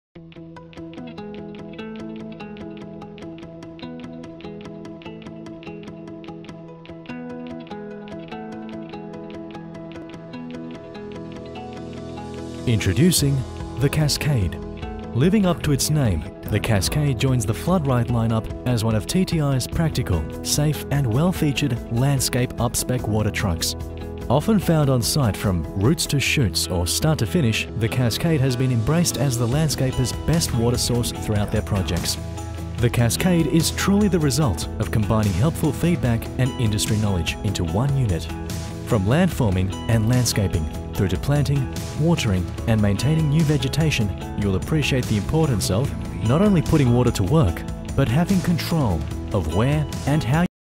Cascade™ » Landscape Spec Water Truck by TTi
Introducing the all new powerful TTi Cascade Landscape Spec Water Truck.

The FloodRite™ 8000L Chassis Mount Water Cart Trucks have been carefully engineered to offer a complete solution for Dust Suppression, soil conditioning and water transfer, that works every time. FloodRite™ is the number one choice for contractors, councils and hire companies across Australia, and boasts a huge range of user friendly functions to help make your life easier. The FloodRite™ range is manufactured entirely by TTi in Melbourne, VIC, and is distributed throughout Australia. Popular truck models include Isuzu & Hino

8000L Heavy duty Polyethylene Water Cartage Tank. Engineer approved tank design for maximum safety
Complies with relevant Australian standards
Food grade polyethylene, fully UV stabilised
20 year tank warranty
Engineer tested Ball Baffle system for safety and stability, eliminates surge
Heavy duty skid mount frame with forklift pockets and lifting lugs
5 year warranty on frame
Diesel or Hydraulic driven 1600L/min pump
5 year warranty on pump
3″ Galvanised Spray bar with 3 x Magnum style heads. Grouped control.
Pipework is hot dipped galvanised, inside and out
Electric over air functions
3″ male camlock discharge port for mass delivery of water via your pump
3″ Suction fill kit, to fill back through your pump quickly, 6 metre suction hose
3″ butterfly valves are fitted on suction/discharge ports for ease of operation
3″ Hydrant fill kit air gap filler with funnel fill
Wireless Remote Controlled, motor on/off, Spray bar on/off. Control all functions from in the cab!
Heavy duty spring rewind hose reel with 20m x 25mm hose and twist type nozzle
Rotating beacon
Sight level tube
Tare weight 1,450kg
Weight when full of water 9,450kg
Options include: Self loading swing up style legs, Dribble bar in cab controlled,